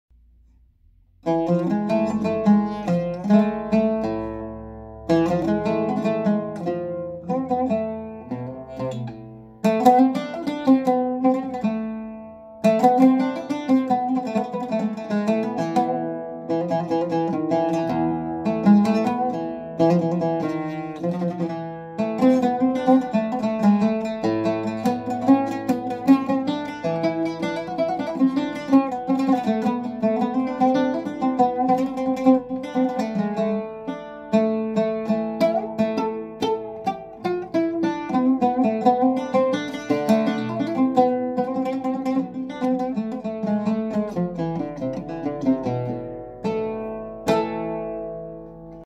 Manol style Ottaman era oud
John Vergara tests his new Manol style Ottoman era oud